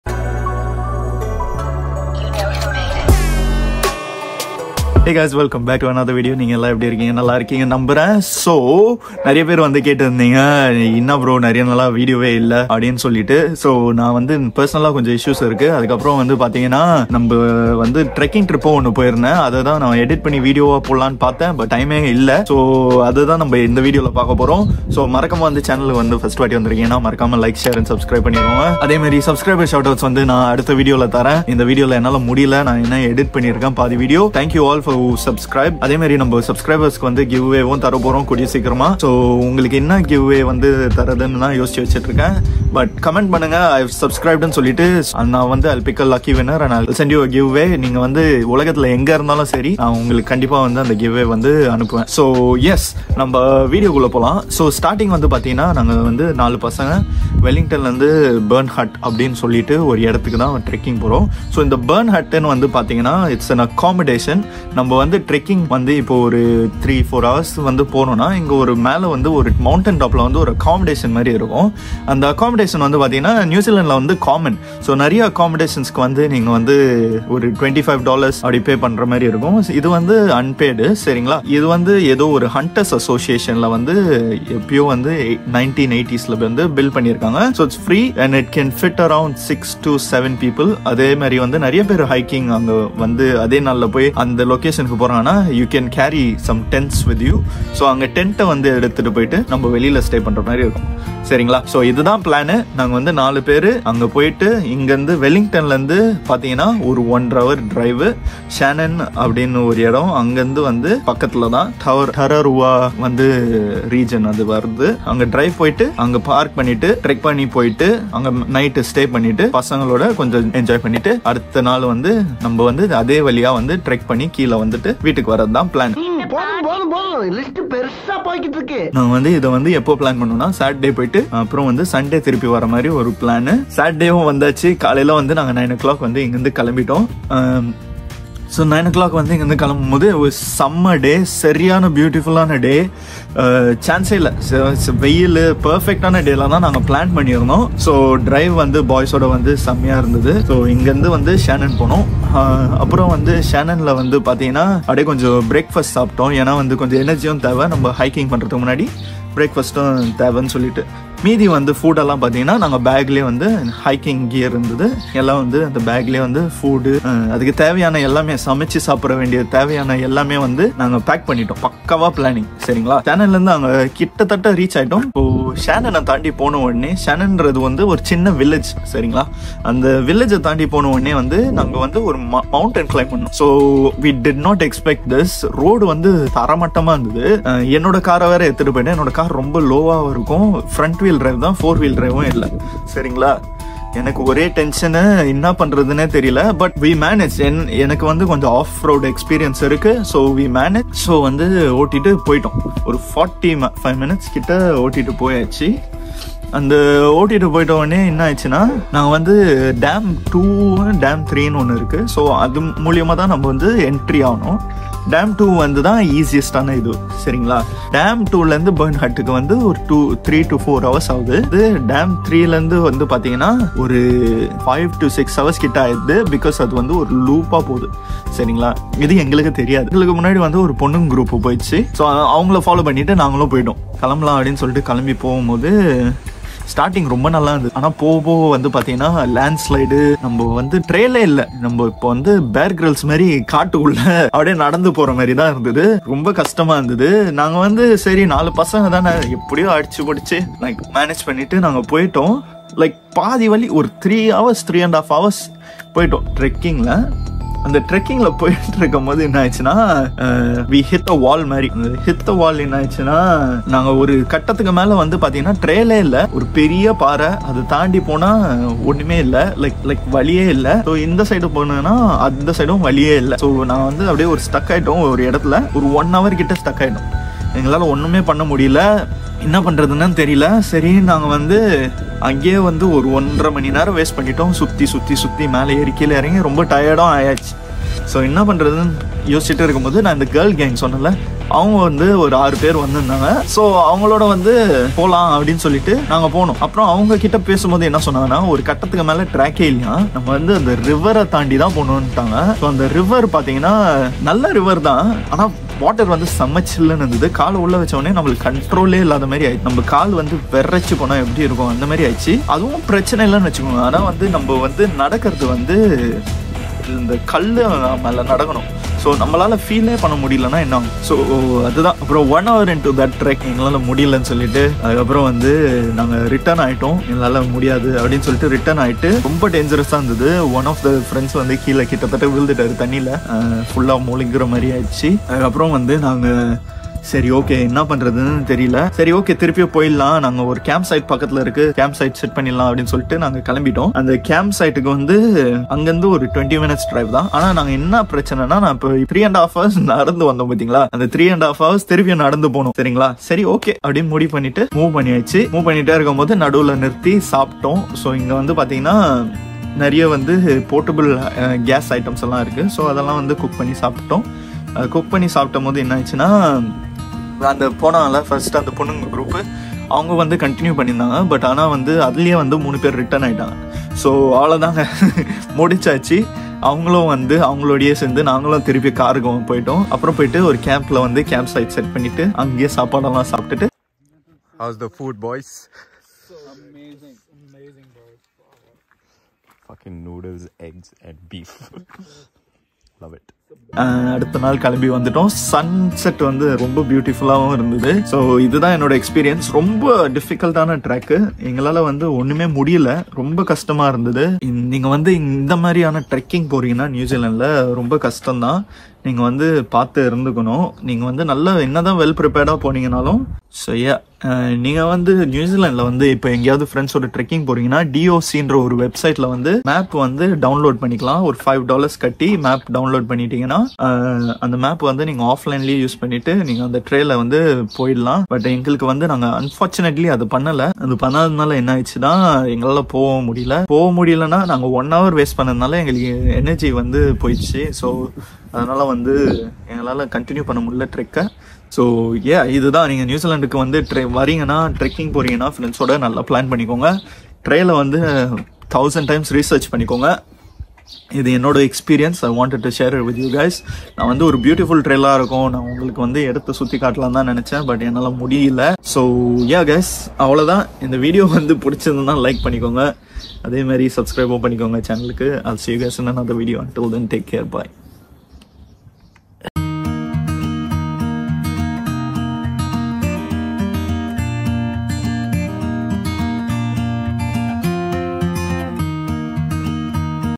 New Zealand-ல Dangerous Trekking trip 😰 | Vanakkam New Zealand
Hey VNZ family!

This video is about my trekking trip with the boys to the Burn Hut which is 104 kms away from where I live. This was one of the toughest trek I have done in my life. We did not complete it due to it being very difficult so we decided to camp instead in the camping sight.

I will see you guys in another video! Until then, take care!

Love you fam!

- VNZ